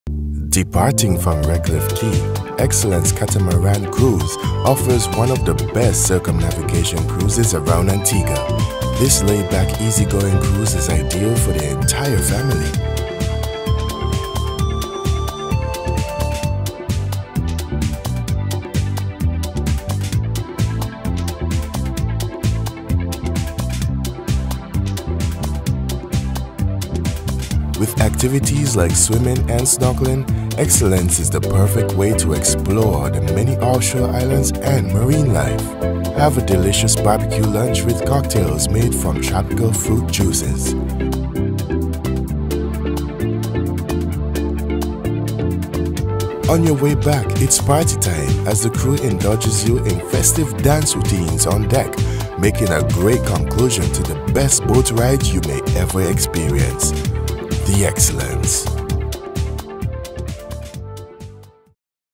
Excellence Catamaran Antigua
The 'Excellence' is Antigua's most exquisite 5 stars, state-of-the-art luxury catamaran.  We welcome all aboard for a full circumnavigation of our island's magnificent coastline. Think you've seen all that Antigua has to offer? Wait till you experience the views by sea!
The sensation you will feel as the alluring coastline glides by with rolling hills shimmering in the distance is one you will never forget.  See areas such as Jumby Bay, Bird Island and the natural spectacle of Devil's Bridge up close and personal.  Be at peace with the world as you gaze at slender coconut palms waving in the crisp breeze and listen to rhythmic calypso & reggae music.
We will whisk you away to a secluded and deserted island called Green Island where you can relax on the beach or explore the nearby reef.  When you get back the friendly crew will cook you up a delicious lunch of, chicken, fish. Drinks are free all day at the open bar.
Next we will cruise across English Harbour, where you will discover Nelson's Dockyard; one of the most prestigious & renowned dockyards in the world.
Before we return to port our journey will take us pass the exotic western coast of Antigua where virgin white sand beaches & pristine bays can still be found. As our captain and crew always say "THIS WILL BE THE BEST THING YOU EVER DO BY FAR IN ANTIGUA, IN THE CARIBBEAN, AND BY EXTENSION IN THE WORLD!!!" and when everyone returns they will all agree that they had a great time onboard the Excellence Catamaran